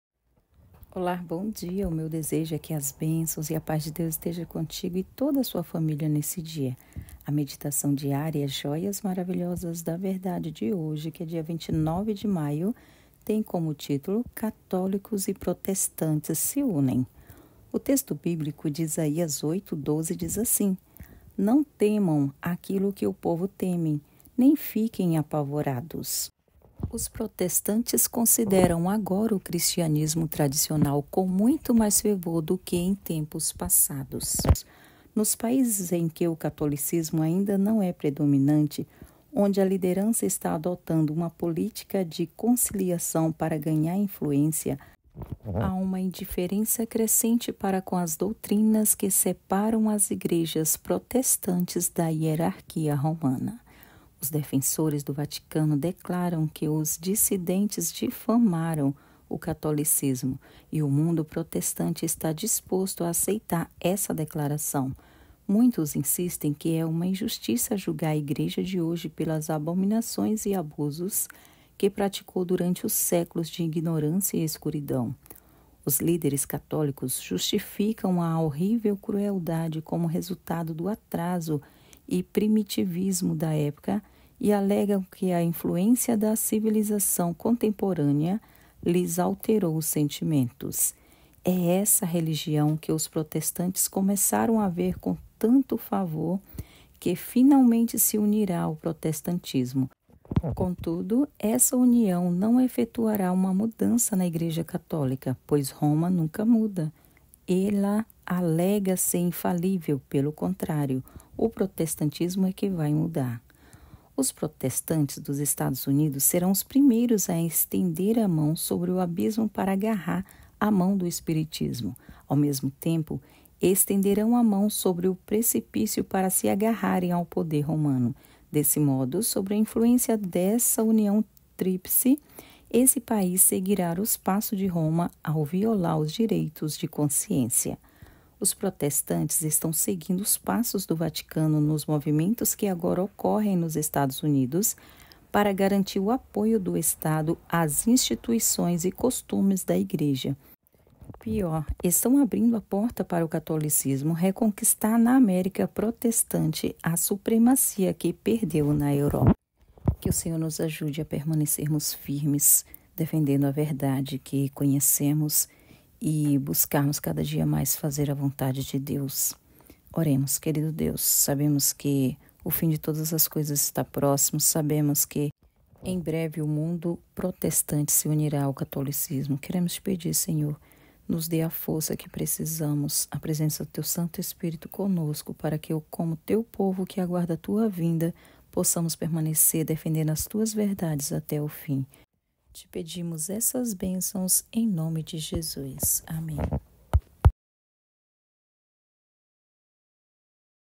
29/05/24:CATÓLICOS E PROTESTANTES SE UNEM - Meditação Jóias Maravilhosas da Verdade
Nunca estivemos tão próximos de ver cumprir-se essa profecia.

Deus tenha misericórdia de nós.